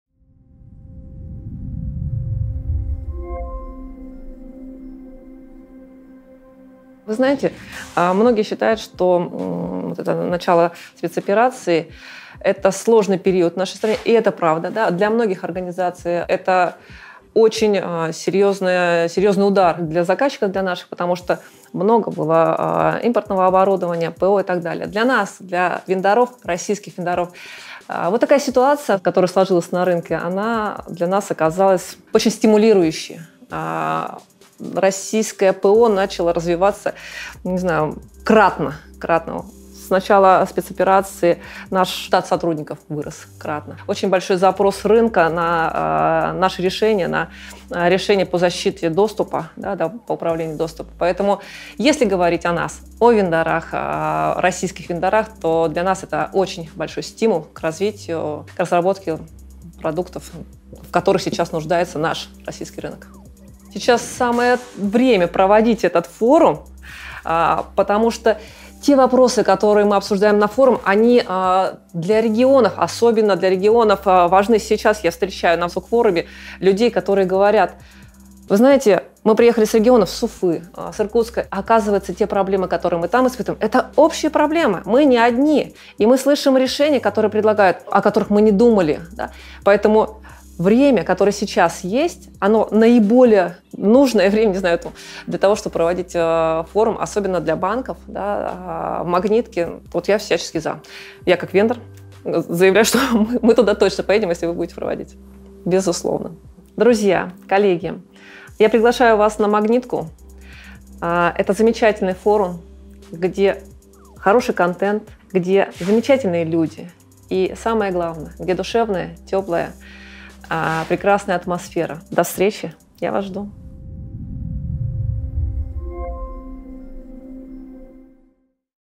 Российское ПО начало развиваться кратно
«Я как вендор заявляю, что мы точно поедем на "Магнитку"»

Ольга Попова, директор по развитию бизнеса компании «Индид», оценила февральский эффект на отечественных ИТ- и ИБ-вендоров как стимулирующий: «Российское ПО начало развиваться кратно». Отметила эксперт и заметное увеличение штата в её компании, а также резкое повышение спроса на продукты «Индид».

Говоря о форуме «Цифровая устойчивость и информационная безопасность России» в ДЦ «Юбилейный», Ольга заявила, что сейчас «самое время» для его проведения — вопросы, которые будут обсуждаться на готовящемся мероприятии, в особенности важны для регионов России. «Магнитка», по выражению спикера, это хороший контент и замечательные люди.

Подробности о мероприятии, его участниках и главных темах постоянно появляются на сайте форума